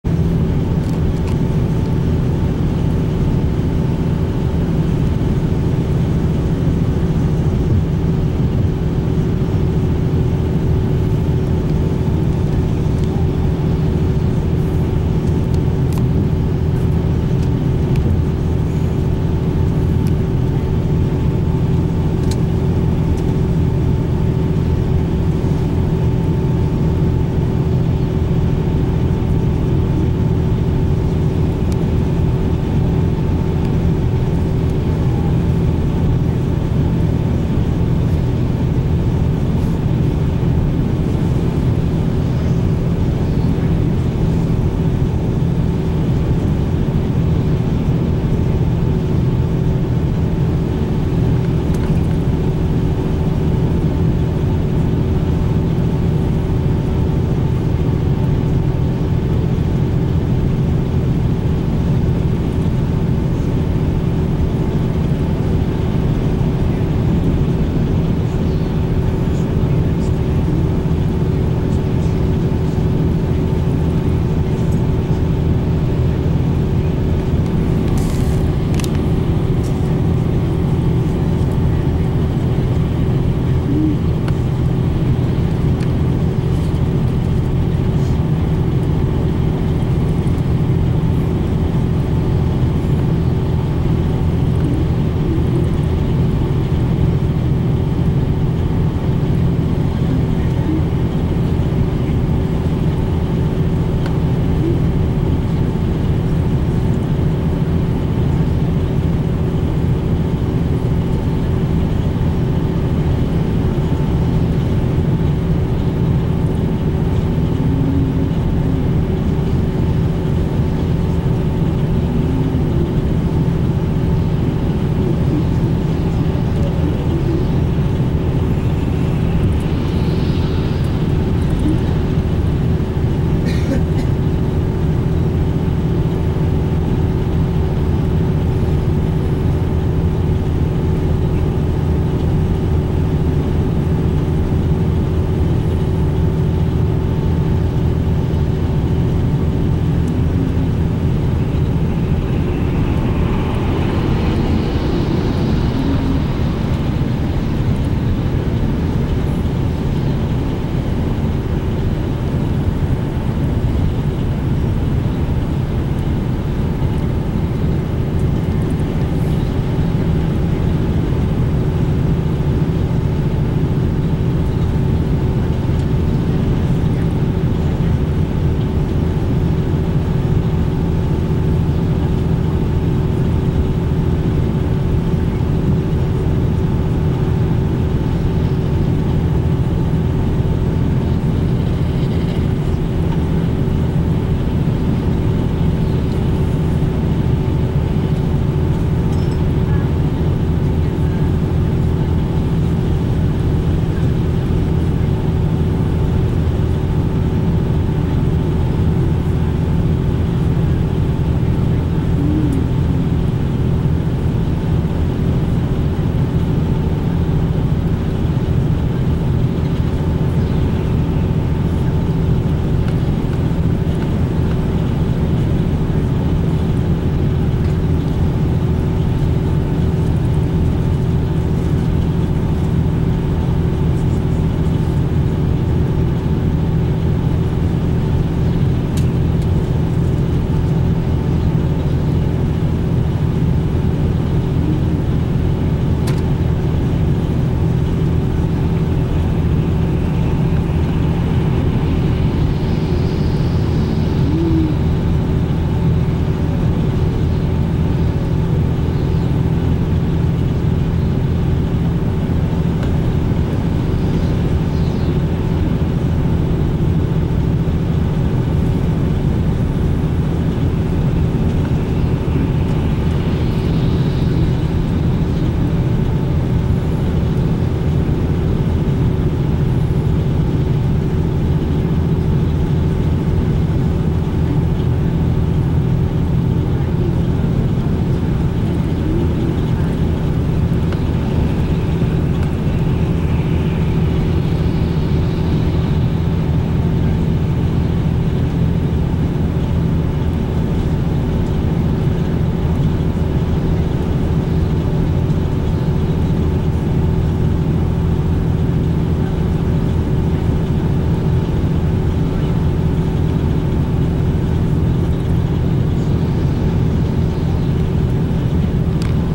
Part 2: Ride on Academy Express CX35 #3513
A ride on Academy Express 2016 CX35 #3513.

Taken: 1/24/2017